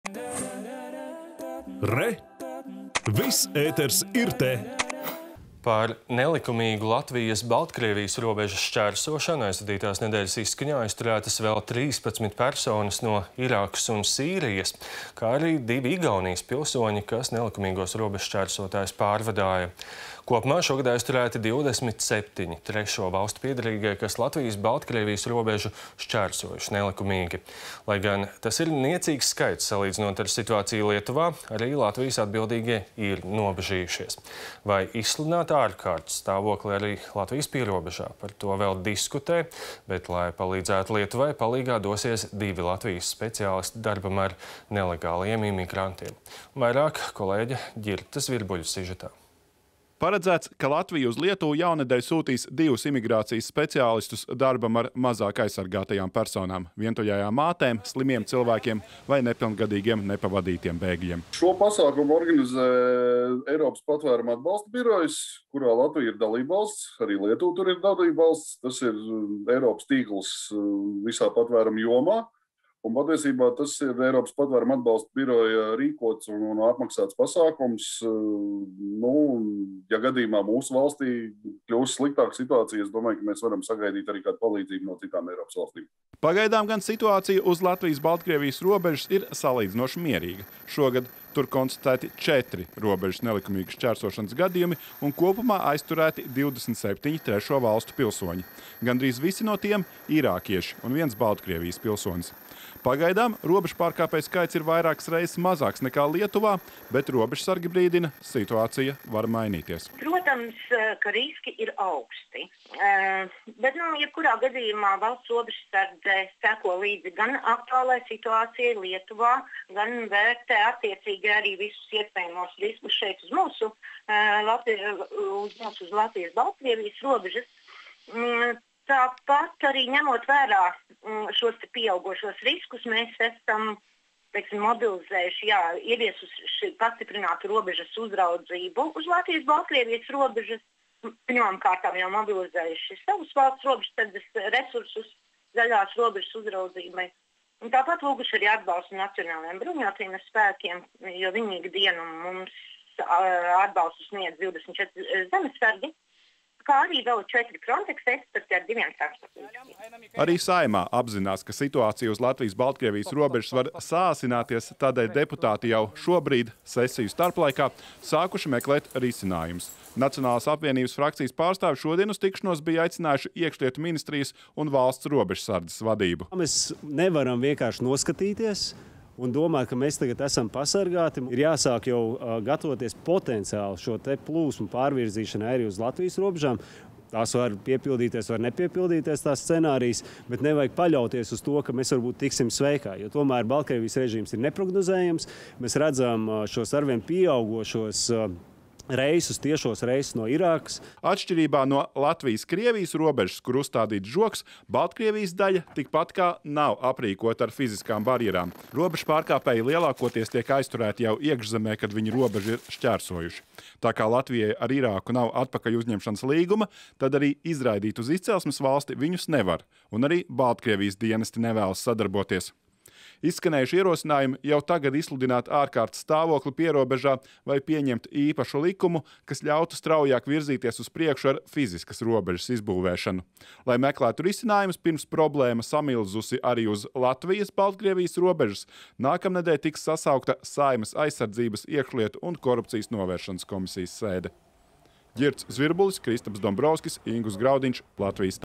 Latvija palīdzēs imigrācijas krīzes skartajai Lietuvai
Par nelikumīgu Latvijas-Baltkrievijas robežas šķērsošanu aizvadītās nedēļas izskaņā aizturētas vēl 13 personas no Irākas un Sīrijas, kā arī divi Igaunijas pilsoņi, kas nelikumīgos robežšķērsotājus pārvadāja. Kopumā šogad aizturēti 27 trešo valstu piederīgie, kas Latvijas-Baltkrievijas robežu šķērsojuši nelikumīgi. Lai gan tas ir niecīgs skaits, salīdzinot ar situāciju Lietuvā, arī Latvijas atbildīgie ir nobažījušies. Vai izsludināt ārkārtas stāvokli arī Latvijas pierobežā – par to vēl diskutē, bet lai palīdzētu Lietuvai, palīgā dosies divi Latvijas speciālisti darbam ar nelegālajiem imigrantiem. Vairāk – kolēģa Ģirta Zvirbuļa sižetā.